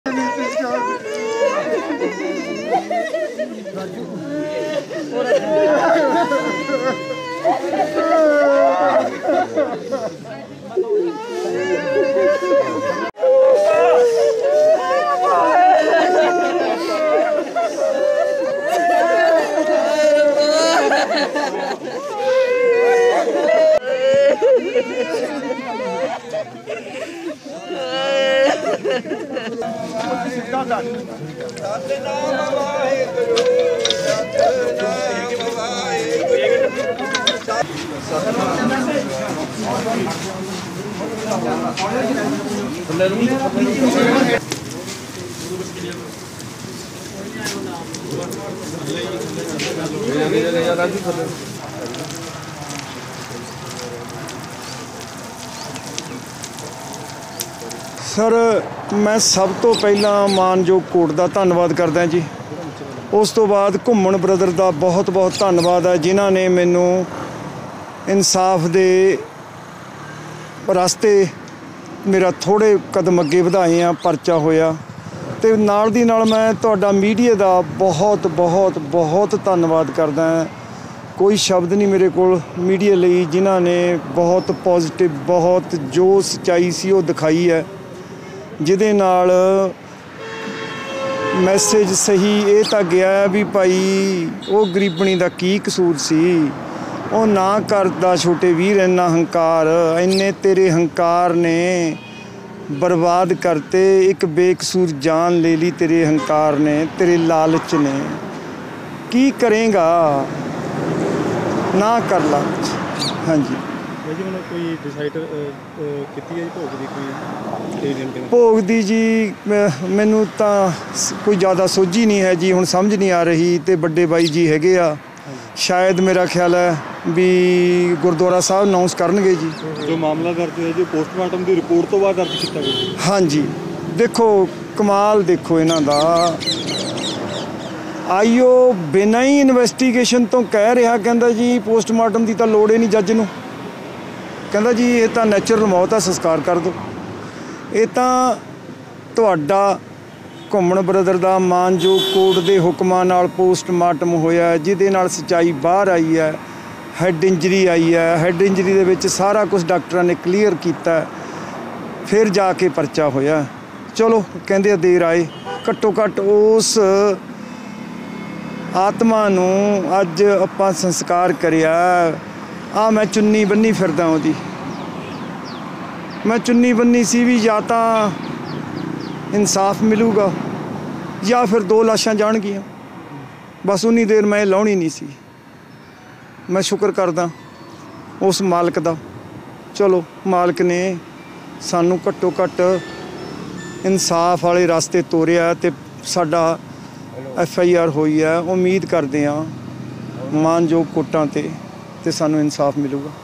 Satwinder Bugga Bhabhi Cremation | ਪਤਨੀ ਦੇ ਪੈਰਾਂ 'ਚ ਲੰਮਾ ਪੈ ਕੇ ਰੋਇਆ ਬੁੱਗੇ ਦਾ ਭਰਾ | Davinder Bhola
Satwinder Bugga Bhabhi Cremation | ਪਤਨੀ ਦੇ ਪੈਰਾਂ 'ਚ ਲੰਬਾ ਪੈ ਕੇ ਰੋਇਆ ਬੁੱਗੇ ਦਾ ਭਰਾ | Davinder Bhola